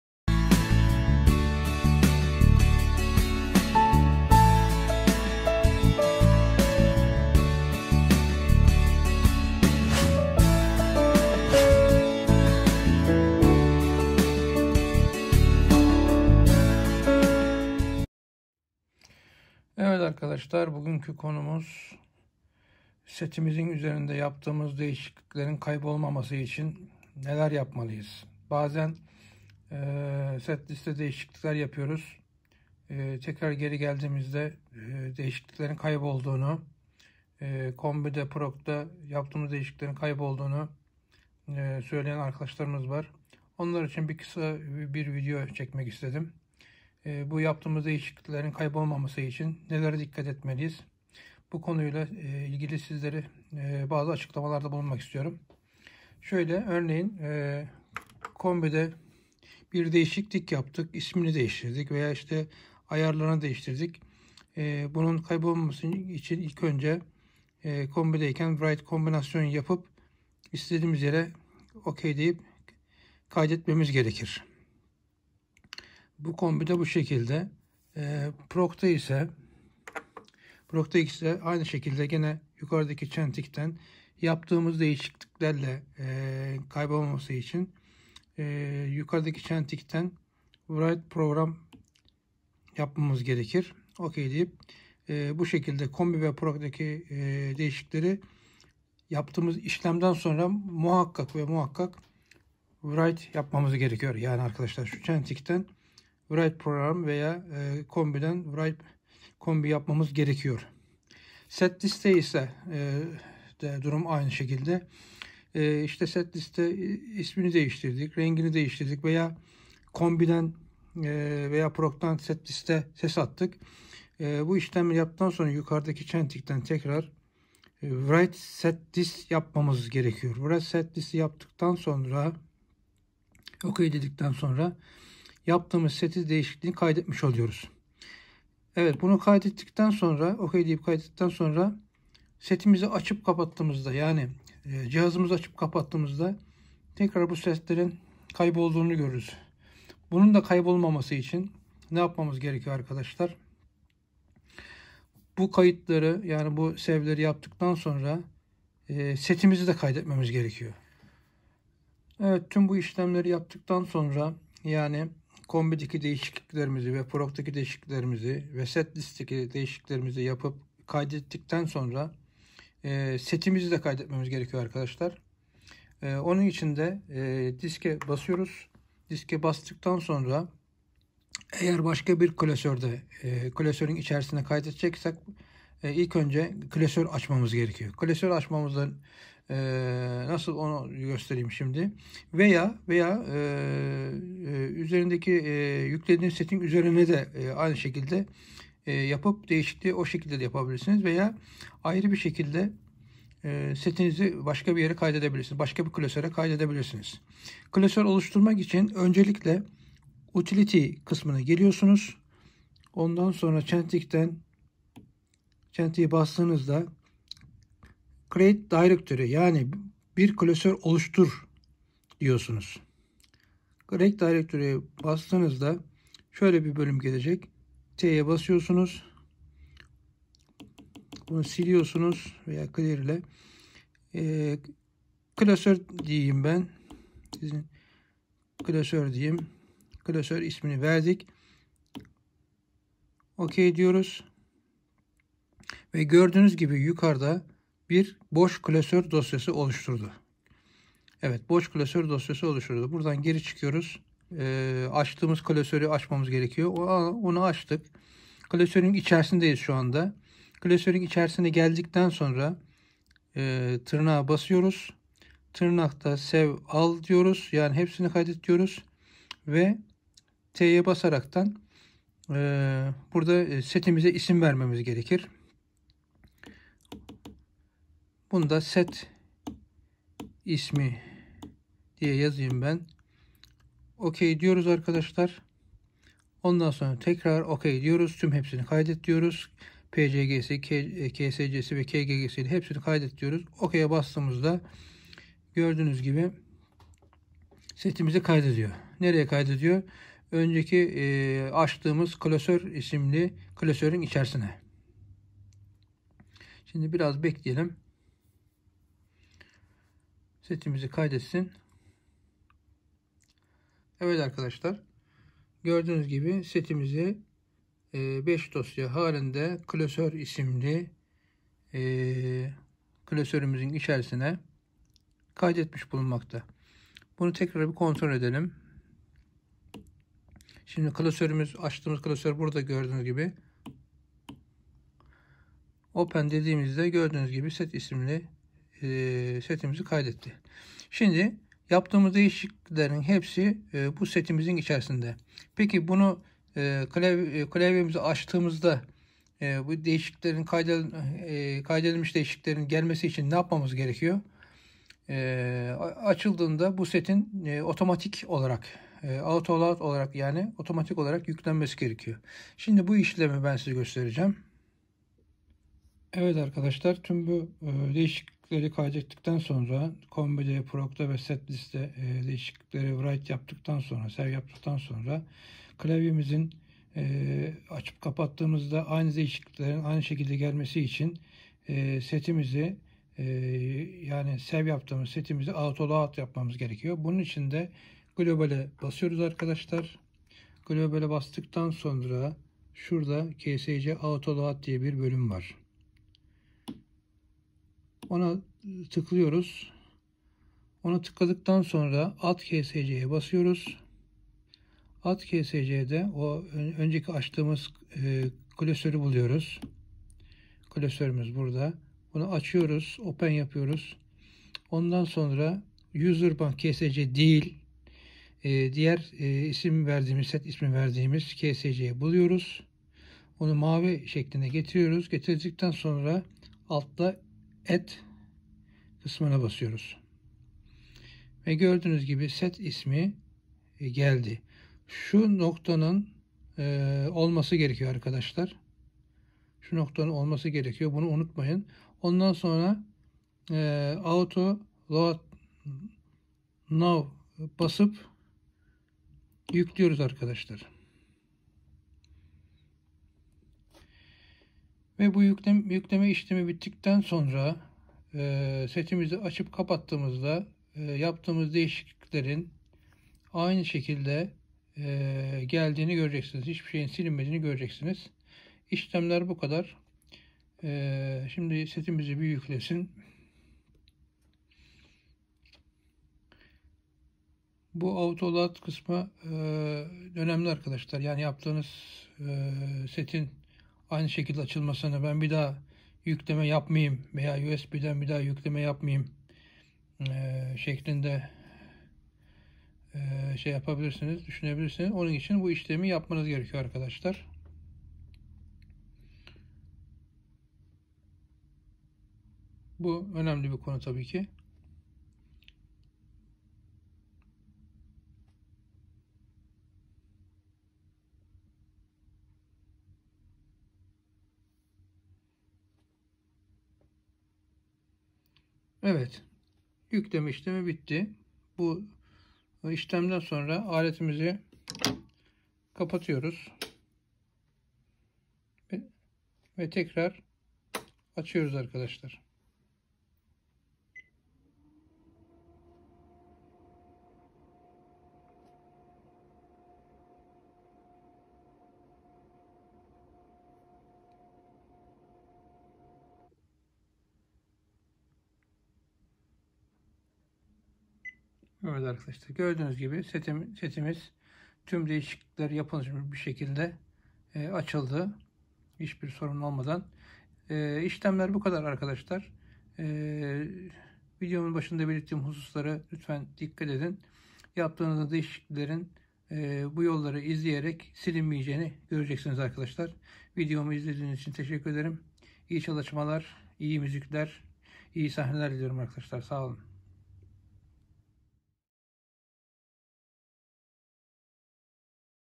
KORG KRONOS SETİNDE YAPILAN DEĞİŞİKLİKLERİN KALICI KAYDEDİLMESİ
Korg Kronos klavyede combi-prog-set list deki yapılan herhangi bir değişikliğin cihazımızı açıp kapattığımızda kaybolmaması için yapılan işlemleri anlatmaya çalıştım.